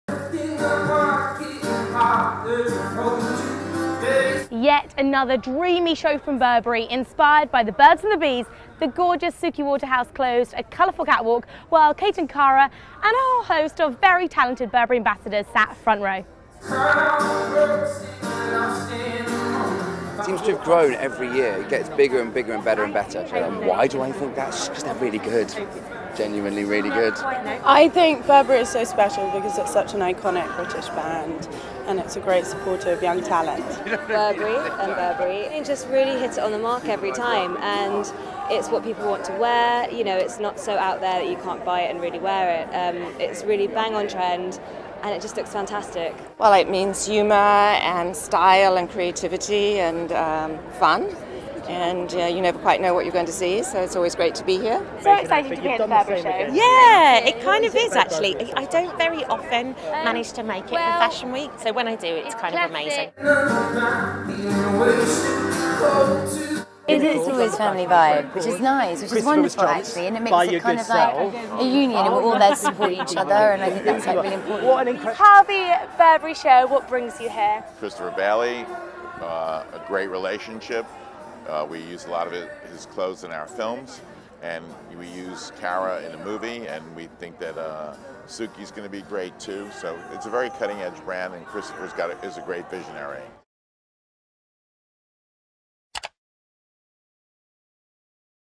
Burberry 2014: Cara Delevingne, Kate Moss, Suki Waterhouse, Jamie Campbell Bower, Cat Deeley
At the Burberry show, Cara Delevingne and Kate Moss arrive together and Suki Waterhouse closes the catwalk at London Fashion Week. Report by Poppy Jamie.

Test